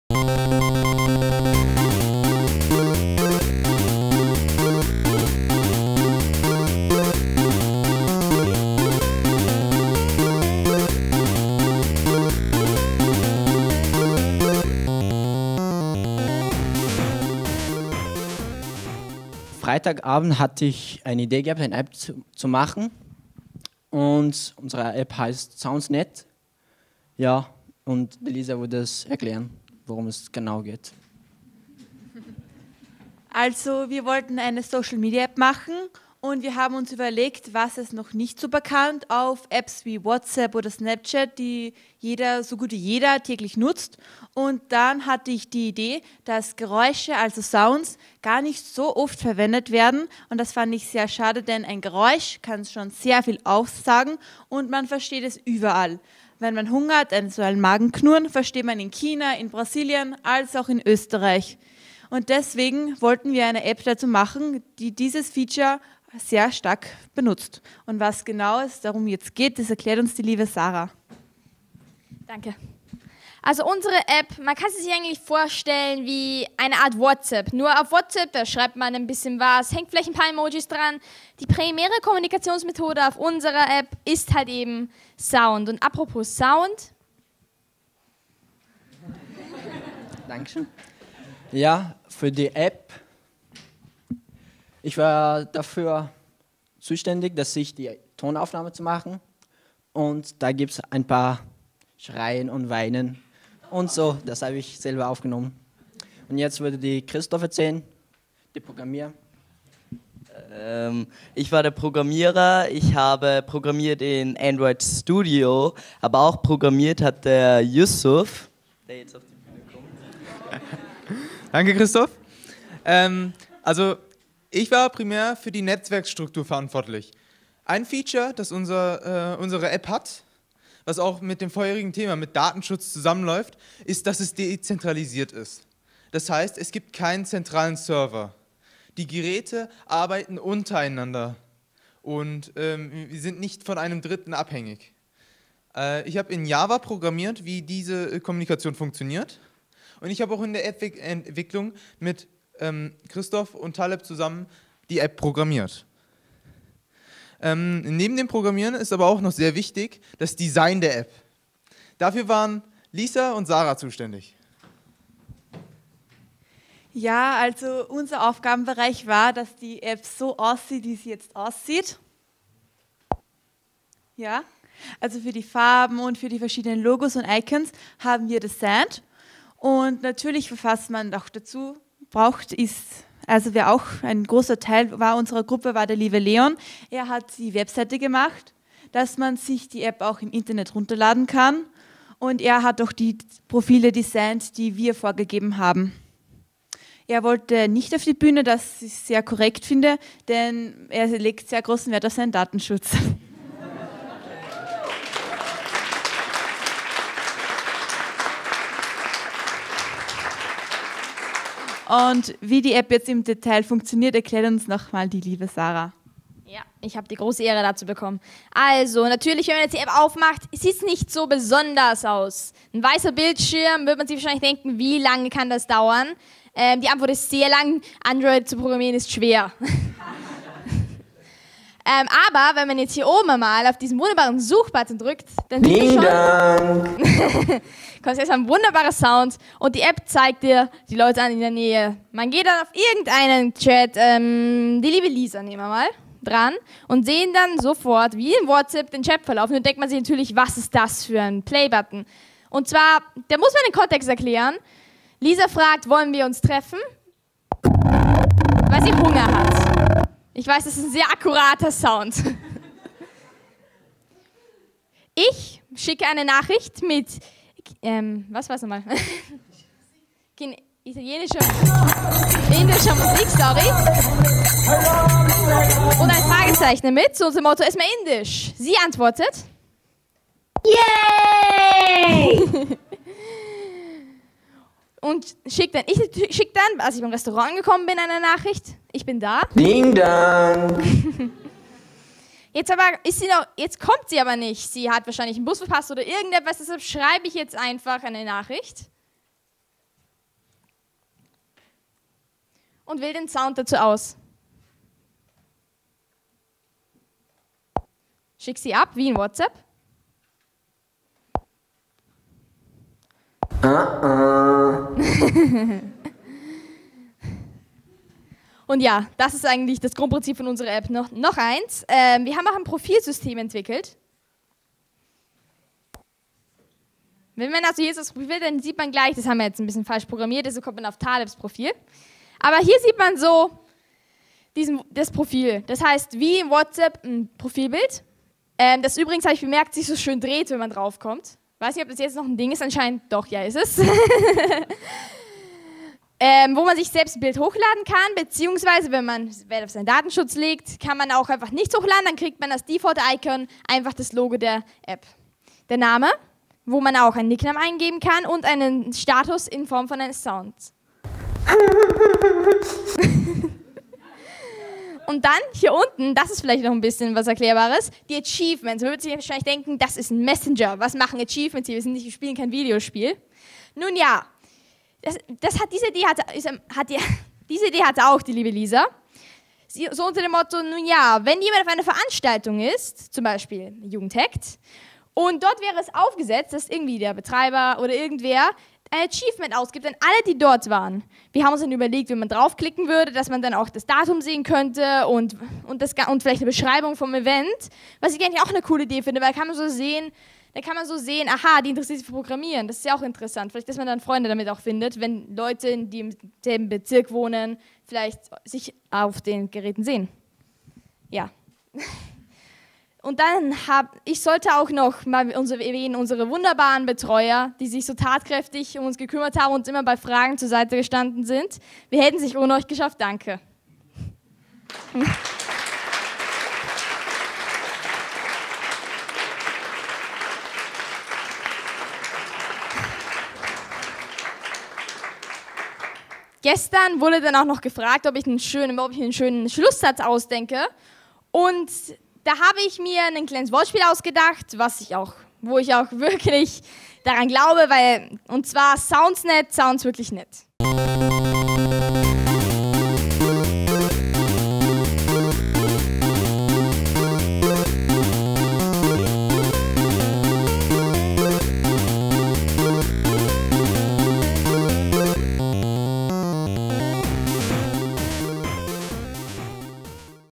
SoundsNett | Jugend hackt AT Linz 2017
SoundsNett ist eine Alternative zu WhatsApp, die ihren Fokus auf Sound und Sprachnachrichten legt. Von Cristoph, Leon, Lisa, Sarah, Taleb und Yusuf.